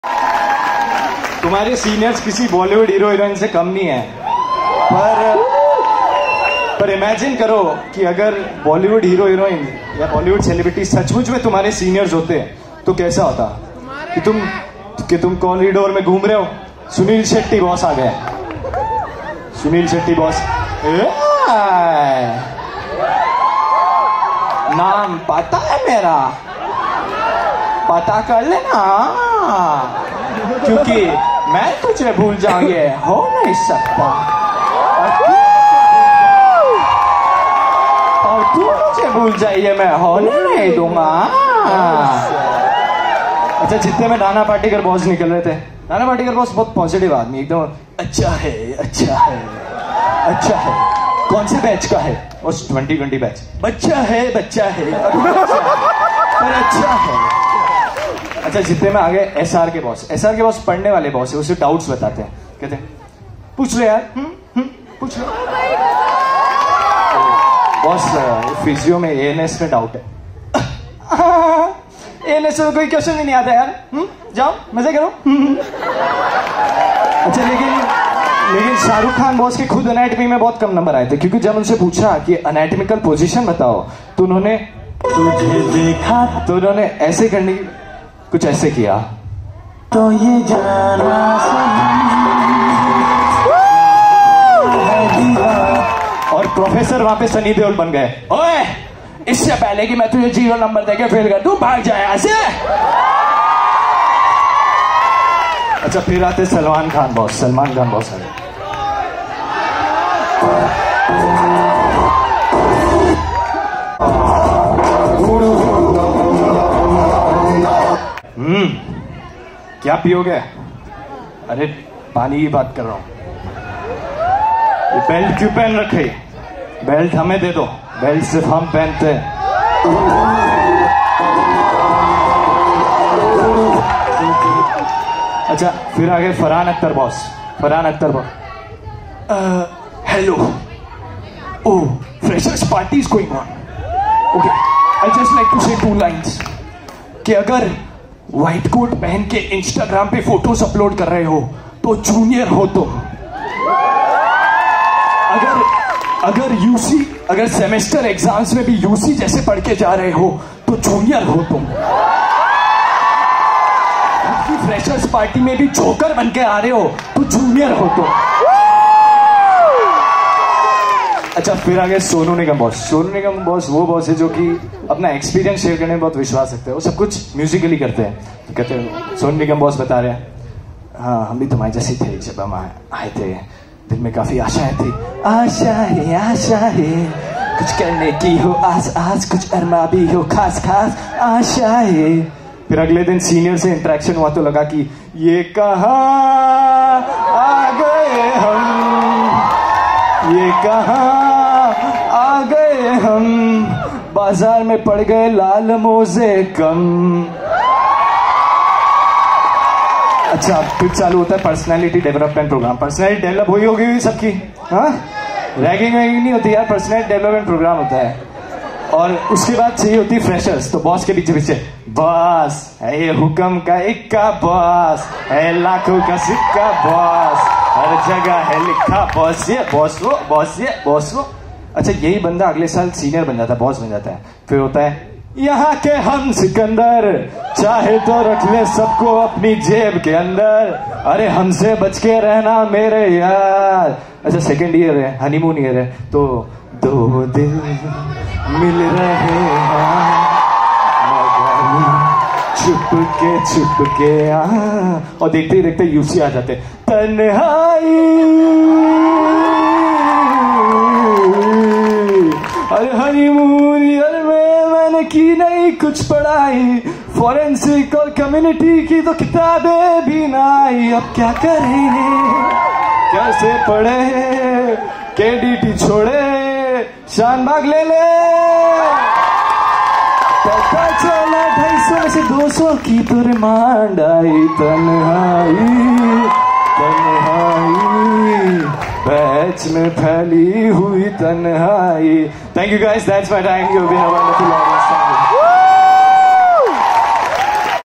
MBBS FRESHERS’ PARTY||STAND UP COMEDY||MIMICRY||IF BOLLYWOOD CELEBRITIES WERE SENIORS||GMCH UDR
Inside jokes of GMCH Udaipur
Uploading just to get embarrassed seeing it 10 years down the line😜

Fresher’s Party 2020 batch - 18/09/2021

Performed by - Kushagra Partani